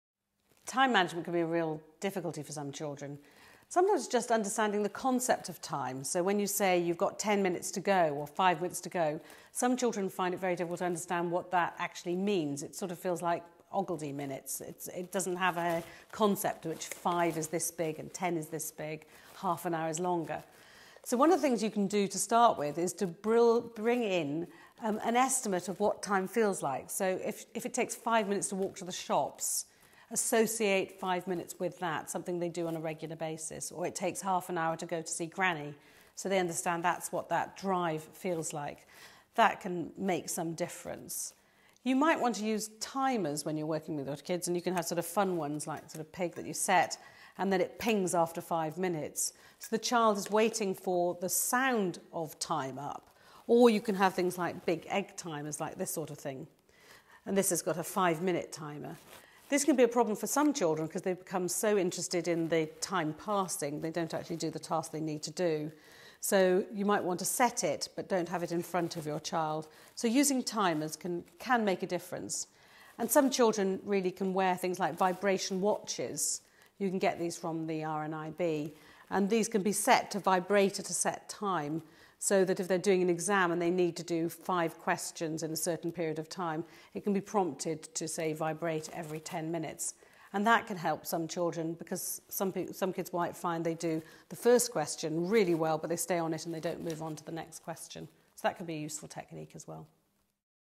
Helping Children With Time Managment
Professor Amanda Kirby discusses how you can help children with time management.